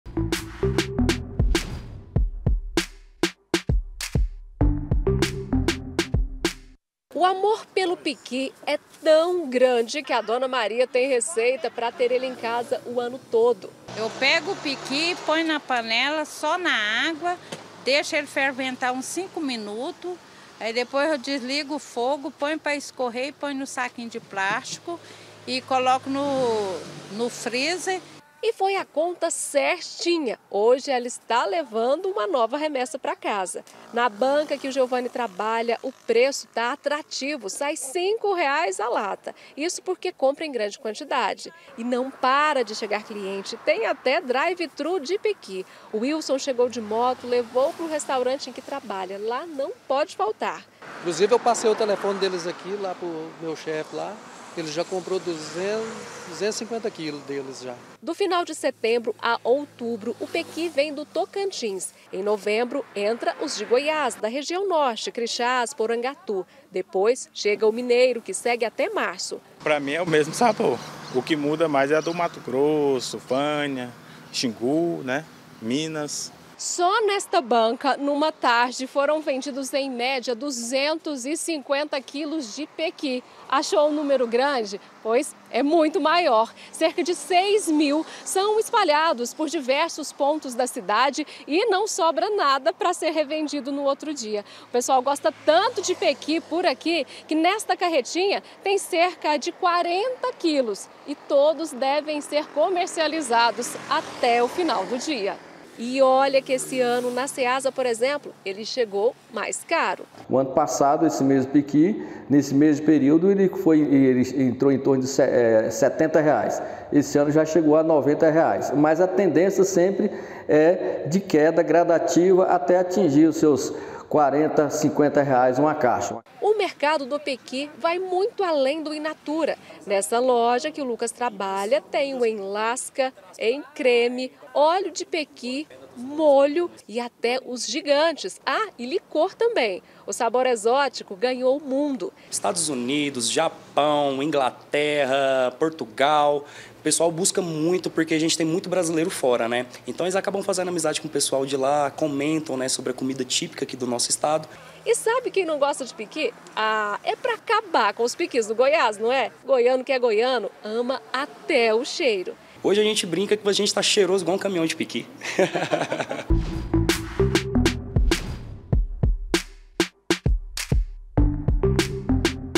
HORA DE GANHAR DINHEIRO COM PEQUI
A COLHEITA DO PEQUI SÓ COMEÇA EM NOVEMBRO, AQUI EM GOIÁS. MAS, JÁ É POSSÍVEL ENCONTRAR PEQUI SENDO VENDIDO NAS RUAS DE GOIÂNIA. TEM VENDEDOR JÁ  COMEÇANDO A GANHAR DINHEIRO COM A TEMPORADA, VENDENDO PEQUI ATÉ PARA FORA DO BRASIL. É O QUE A GENTE VAI VER NO QUADRO MERCADO EM MOVIMENTO.